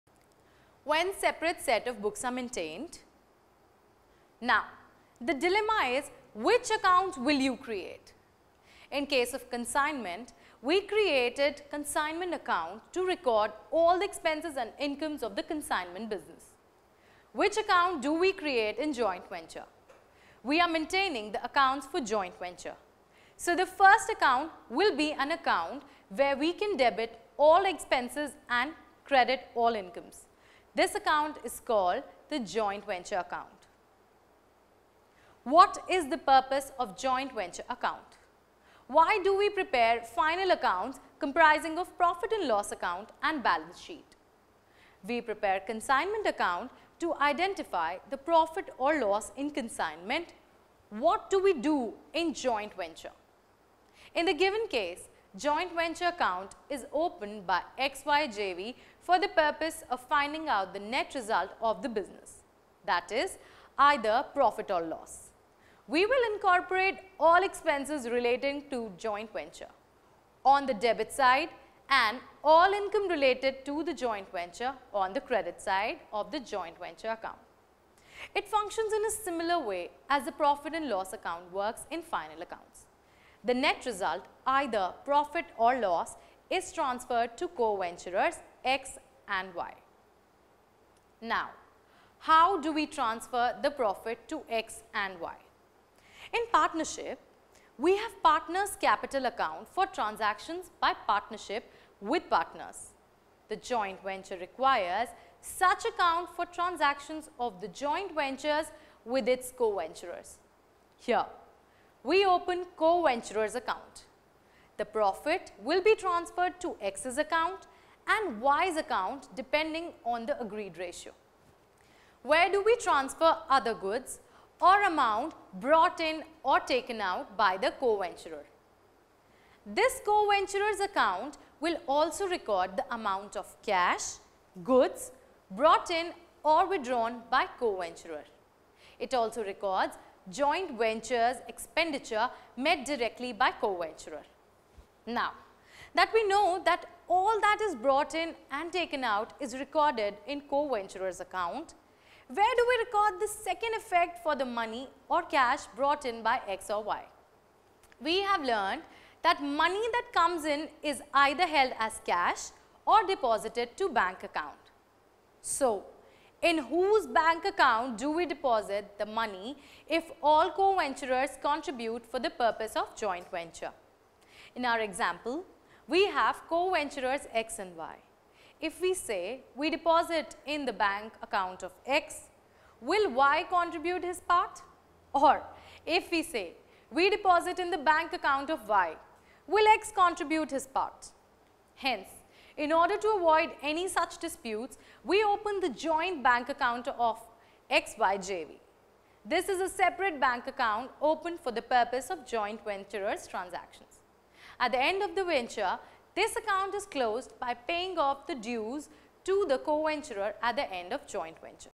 Accounting for Joint Venture When separate set of books are maintained
This video takes you through the accounting procedures of Joint Venture. Learn the accounting effects when separate set of books are maintained. This is a part of our video lecture series on the CPT course.